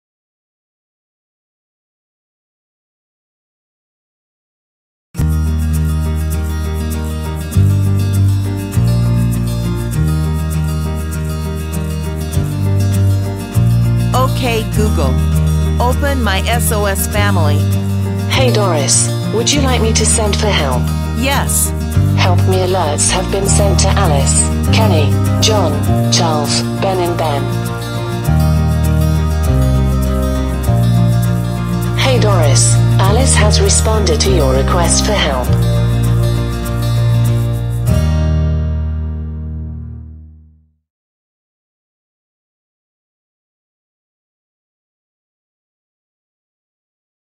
Google Home Voice App - My SOS Family
Emergency SOS App for Google Home Smart Speakers.
Use your voice and any Google Assistant enabled device together with the MY SOS FAMILY app to send SOS alerts to Family, Friends and Neighbours by SMS, Automated Phone Calls and Email.

Search for the MY SOS FAMILY App on Google Home App and set-up an account

Works with Alexa enabled devices too.

Ideal for vulnerable people who spend time alone at home.
to find out about more emergency sos products.